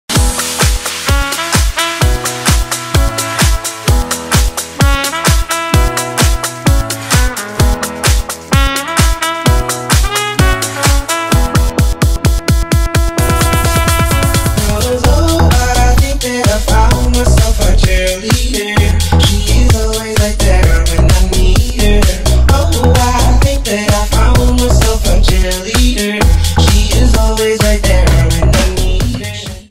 Cours de Zumba
Cours de Zumba à Magic Form Bordeaux - Club de remise en forme